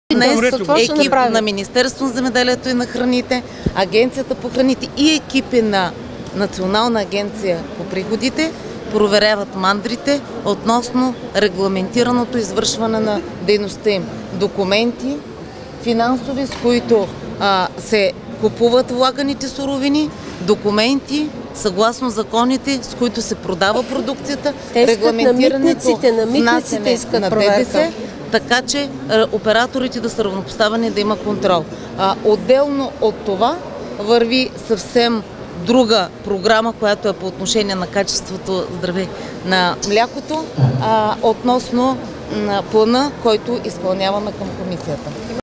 Десислава Танева: Агенцията по безопасност на храните и данъчните проверяват мандрите
Десислава Танева: Агенцията по безопасност на храните и данъчните проверяват всички мандри в страната

ilko gruev